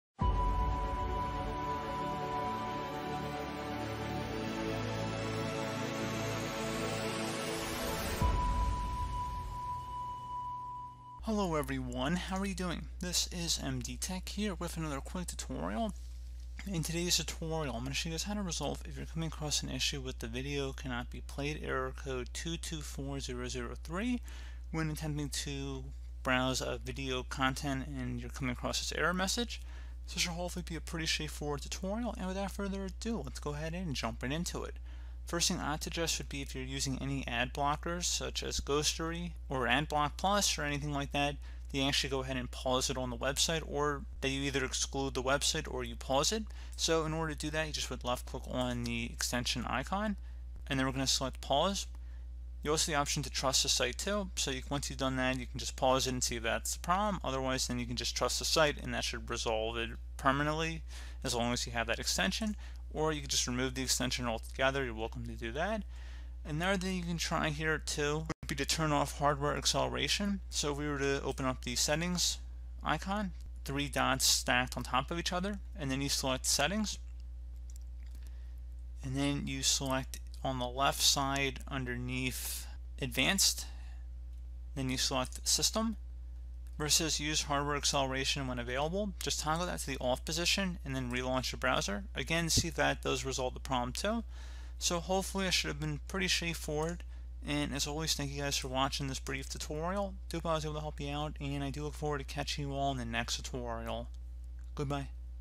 Không thể phát tệp video này, Mã lỗi 224003
[Khắc phục] Không thể phát tệp video này Mã lỗi 224003.

Mã lỗi 224003 gặp phải khi người dùng cố gắng phát video trên các Trình duyệt web phổ biến. Lỗi này thường xảy ra trên JW Player, đây là Media Player được nhúng phổ biến nhất với 20 tỷ luồng hàng tháng. Có thể có nhiều lý do khác nhau khiến trình duyệt không thể phát video, do đó, điều quan trọng là phải chẩn đoán nguyên nhân gốc rễ của sự cố trước tiên.

Điều tra vấn đề, chúng tôi đã phát hiện ra có thể có một trong những lý do sau đây cho Mã lỗi 224003:

Một quy trình khác đang chạy trên trình duyệt web đang chặn video
Cài đặt tùy chỉnh của trình duyệt của bạn có thể là nguyên nhân khiến video bị chặn
Bạn có thể đã thêm Tiện ích mở rộng hoặc Tiện ích bổ sung của bên thứ ba trên trình duyệt của mình
Trình duyệt web của bạn hoặc Trình phát phương tiện được nhúng không cập nhật
Có thể có sự cố kết nối trên hệ thống của bạn

Trước khi tiếp tục giải pháp, bạn cần đảm bảo rằng Trình duyệt web của bạn được cập nhật, không có sự cố kết nối nào trên hệ thống của bạn.

Hướng dẫn này sẽ áp dụng cho máy tính, máy tính xách tay, máy tính để bàn và máy tính bảng chạy hệ điều hành Windows 10, Windows 8 / 8.1, Windows 7. Làm việc cho tất cả các nhà sản xuất máy tính lớn (Dell, HP, Acer, Asus, Toshiba, Lenovo, Samsung).